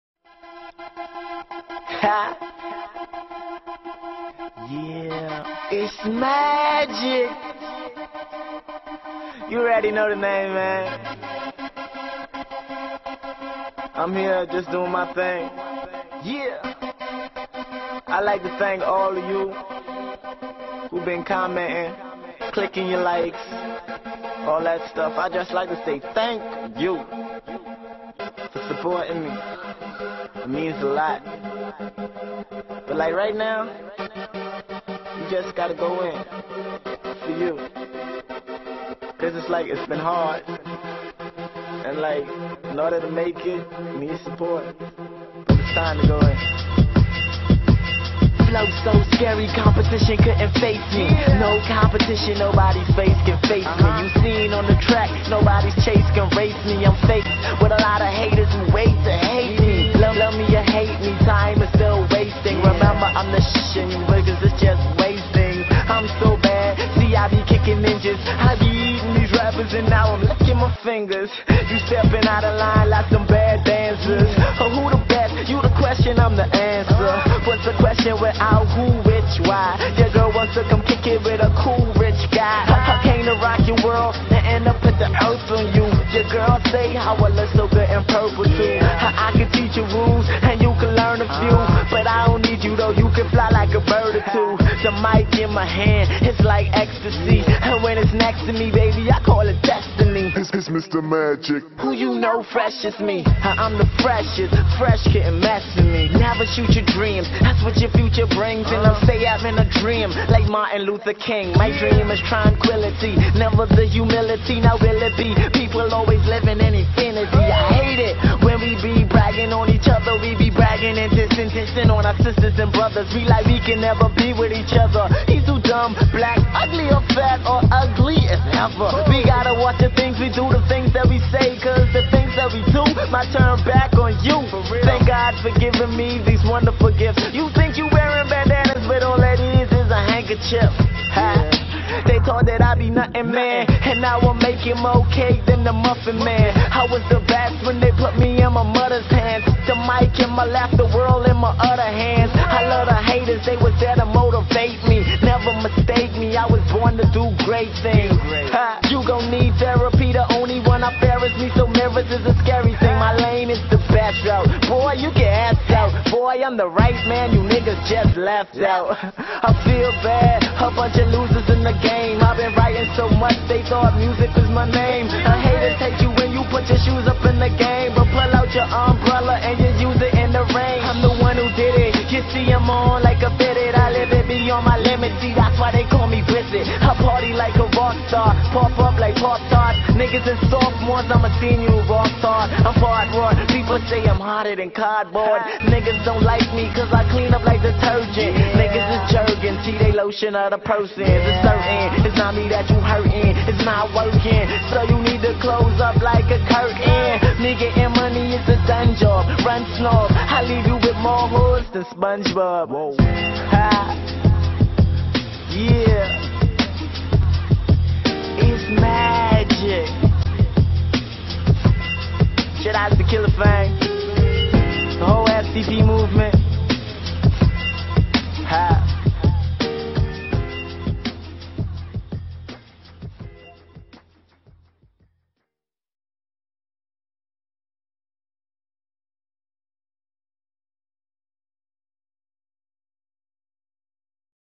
DIrty Money Studio Video - Wizzard
A freestyle what i did For the People Who thought they wasnt good enough to do sumthing anythingg you put your mind too you can do (THE PERSON wHO wROTE THIS DID THIS) ALL YOU GATAA DO IZ BELIEVE!!!